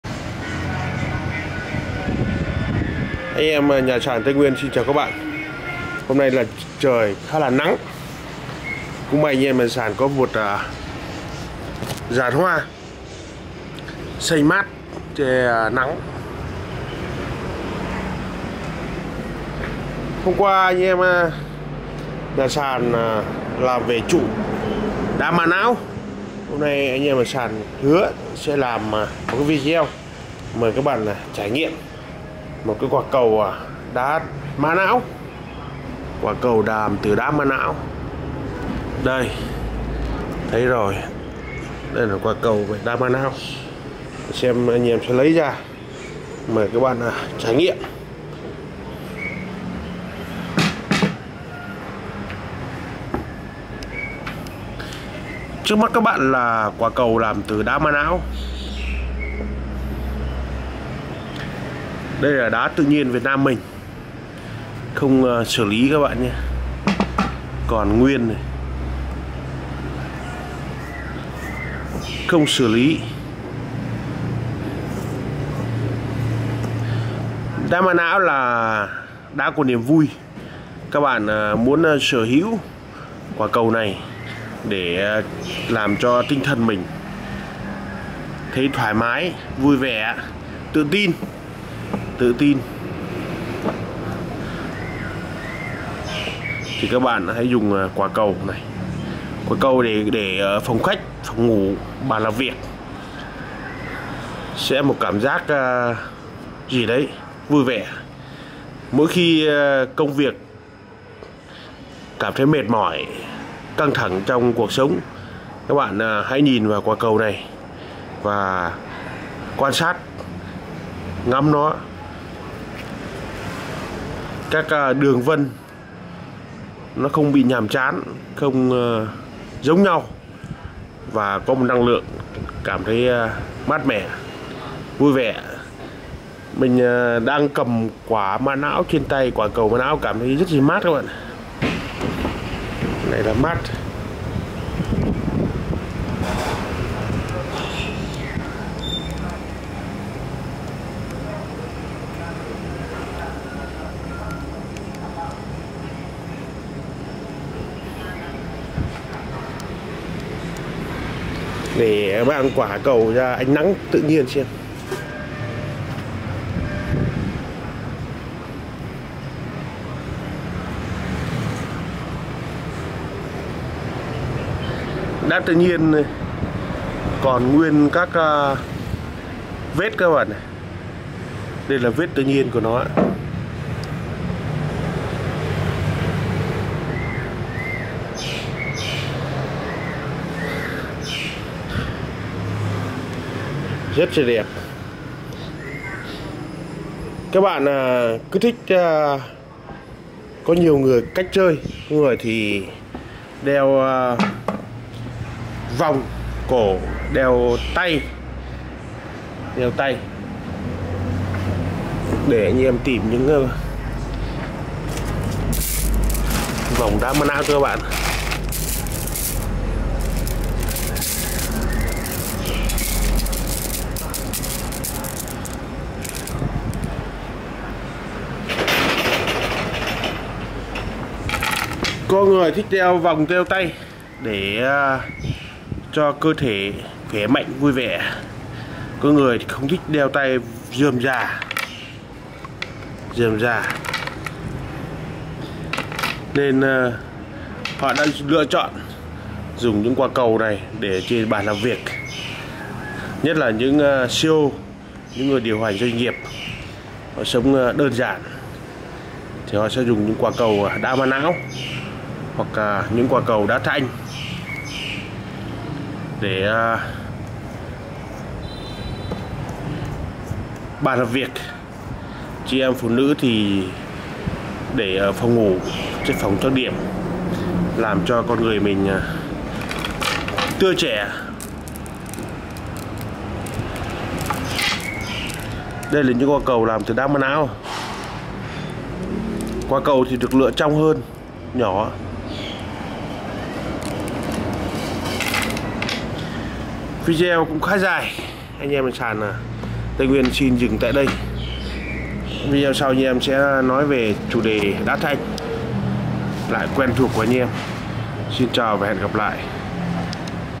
Quả Cầu Đá Mã Não Việt Nam Có Sức Mạnh, Cảm Hứng Vui Vẻ
Quả cầu đá mã não
quả cầu phong thủy đá mã não (agate) là một trong những vật phẩm phong thủy làm mưa làm gió thị trường phong thủy Việt Nam. Do đã được khai thác mạnh những năm trở lại đây nên nguồn đá ngày càng trở nên khan hiếm.
Cùng tìm hiểu xem Quả cầu phong thủy đá mã não có ý nghĩa như thế nào trong phong thủy bạn nhé !
Đôi nét về đá mã não ? Quả cầu phong thủy đá mã não là gì ?